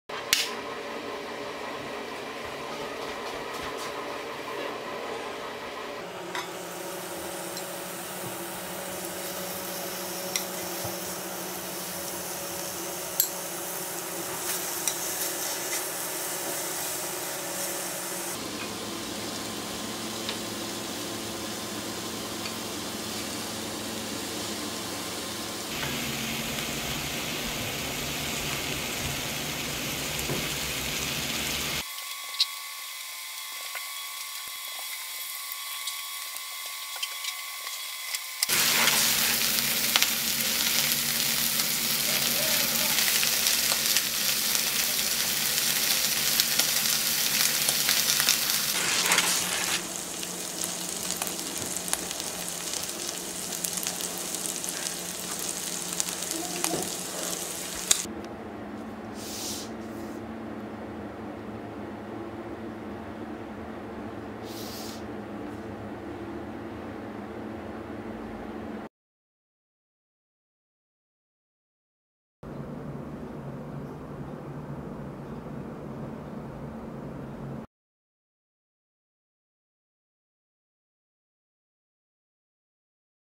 대구 막창 ~~!!! 굽는소리까지 기가막힌다~!!!
대구에서 생산되는 대구 명물 대구 막창 간단하게 조리해서 드실수 있게 알맞은 포장으로 소개 드려봅니다~ 그럼 한번 만보실까요?